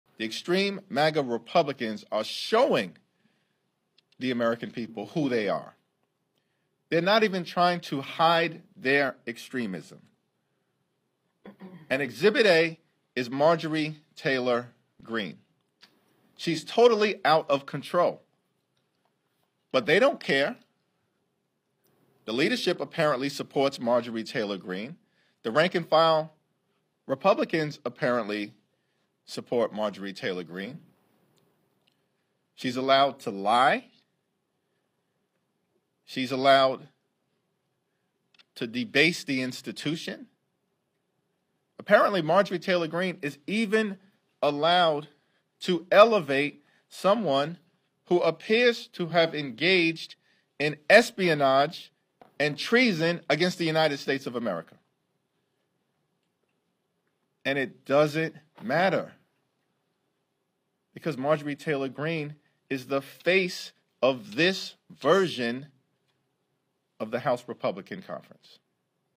Jeffries Slams Marjorie Taylor Greene: 'She's Totally Out Of Control'
Rep. Hakeem Jeffries called Rep. Marjorie Taylor Greene "the face" of "extreme MAGA Republicans" in the House. "She's allowed to lie, she's allowed to debase the institution," the House minority leader said Thursday. "She's totally out of control."